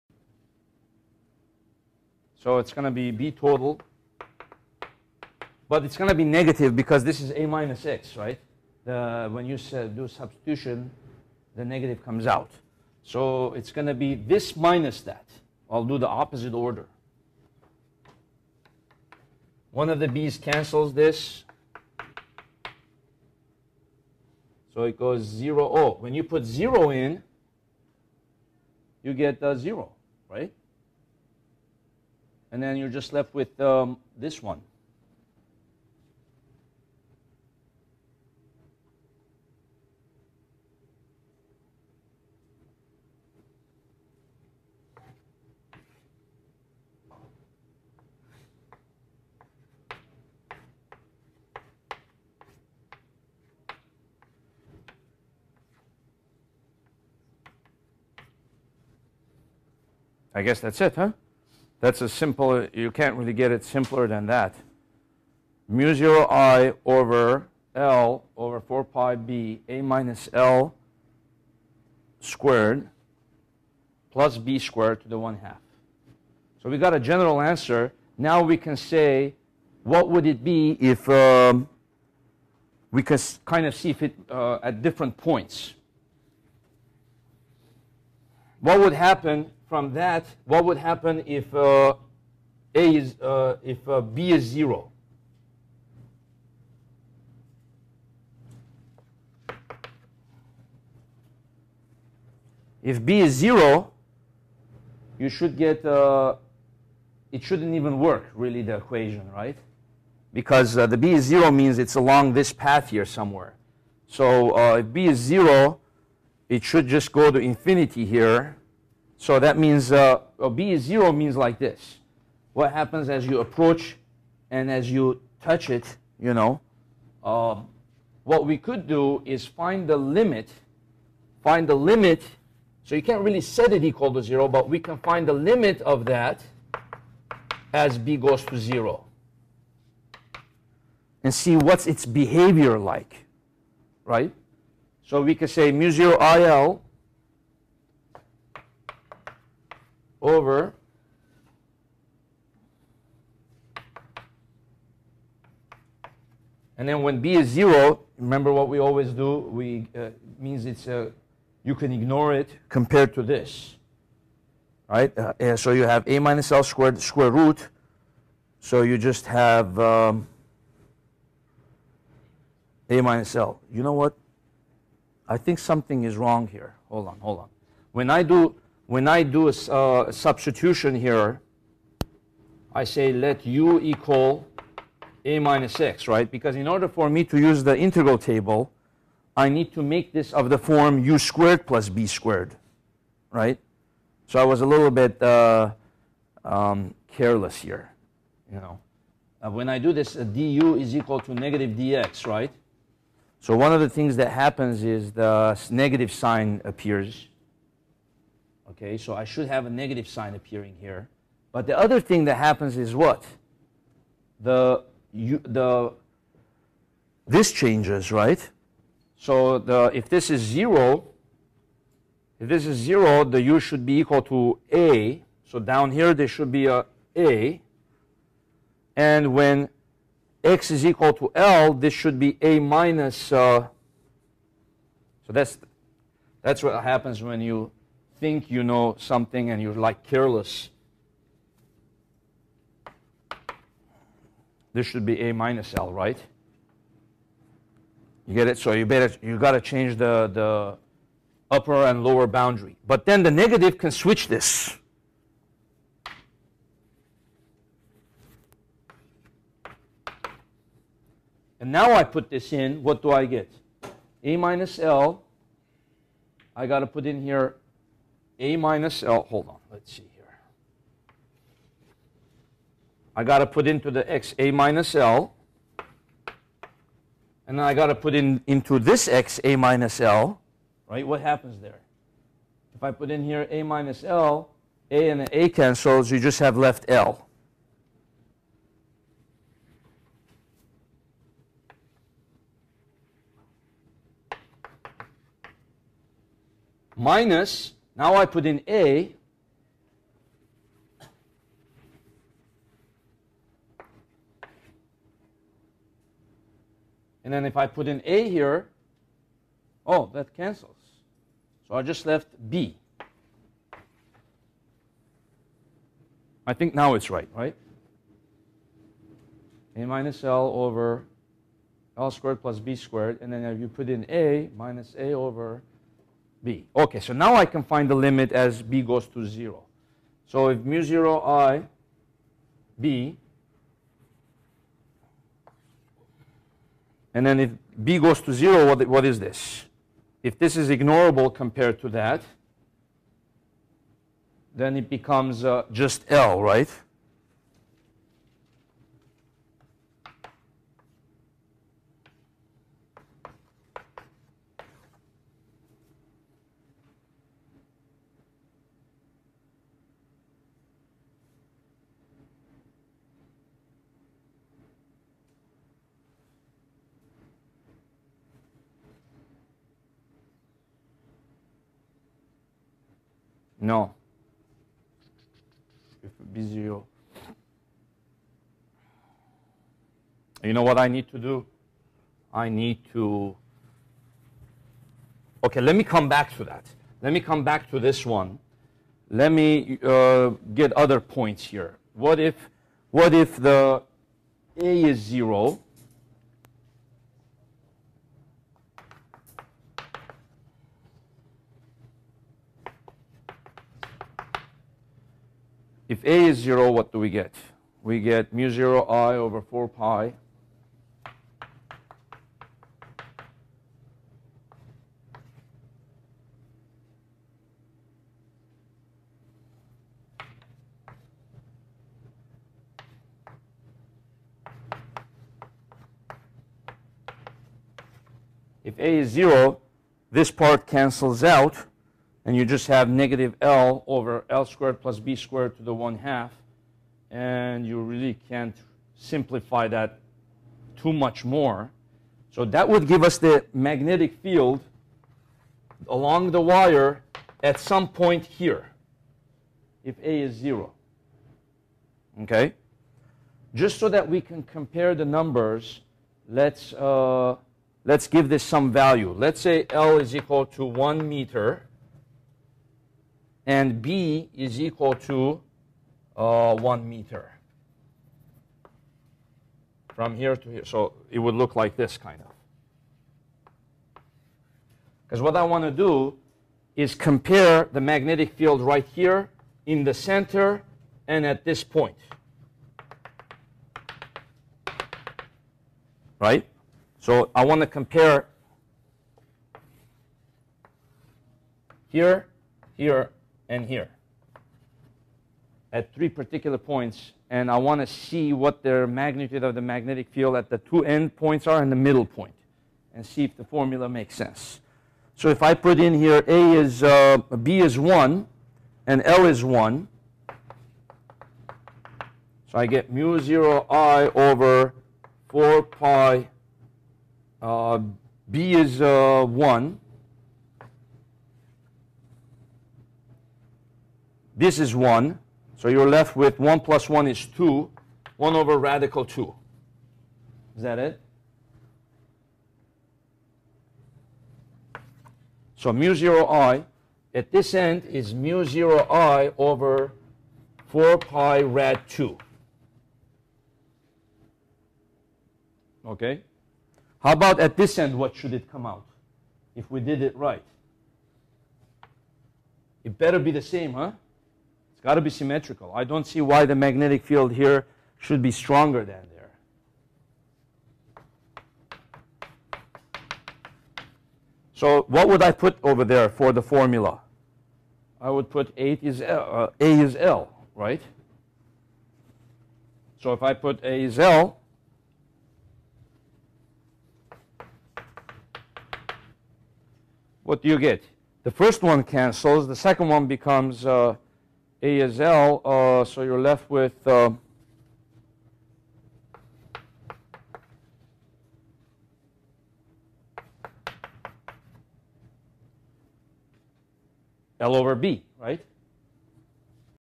Magnetic Fields Example 1 Part 2
This clip illustrates how to use the Biot-Savart Law to find the magnetic field at any point around a straight current carrying wire of a certain length. (Part 2)